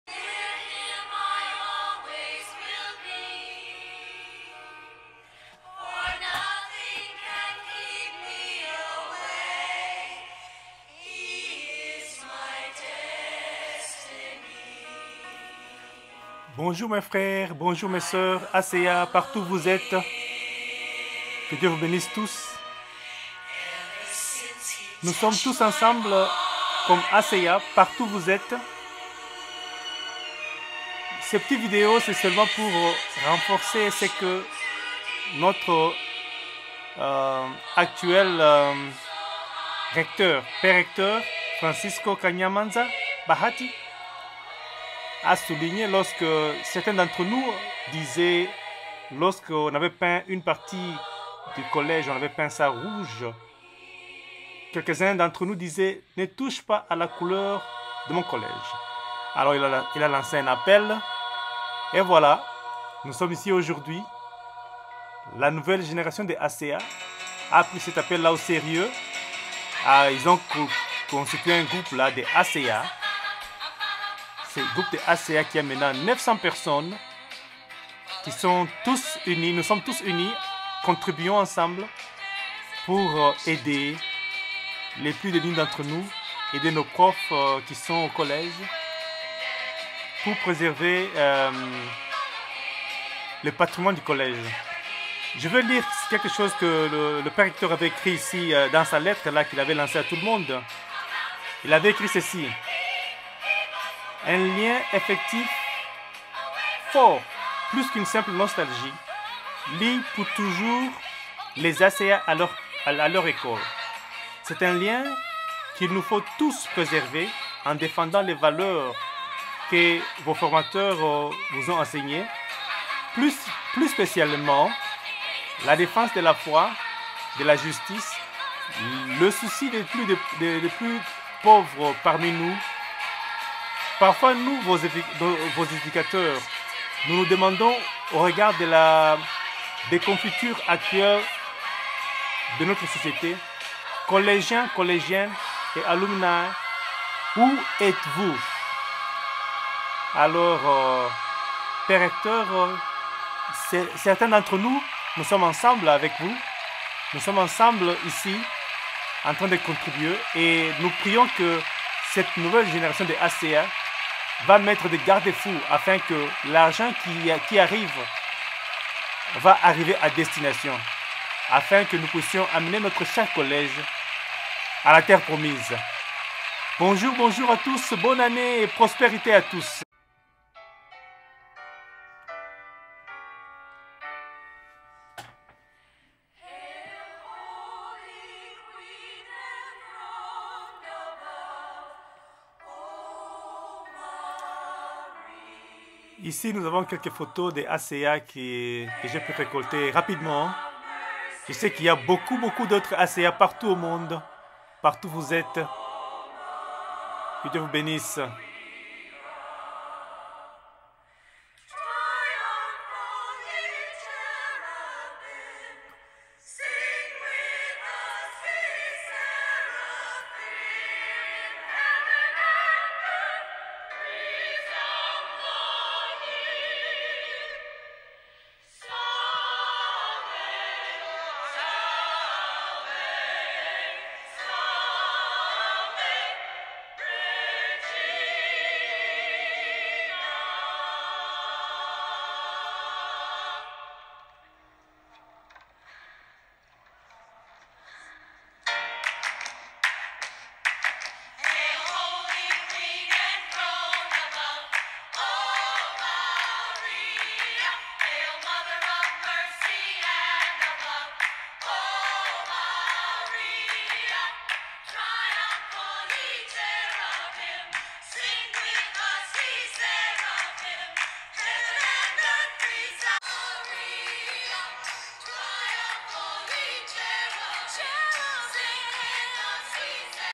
College Alfajiri Bukavu appelle les ACA
College Alfajiri construit en février 1938, le pères jesuits on investit beaucoup au College. Un point touristic a Bukavu. Pere recteur Francisco Kanyamanza Bahati lance un appelle de l'aide a tous les ACA (Ancien du College Alfajiri), internat, basketball, football, College notre dame de la victoire, école maternelle, primaire, secondaire, centre de spécialisation, internat, Au college se forge en toi l'homme/la femme de demain, Stella Duce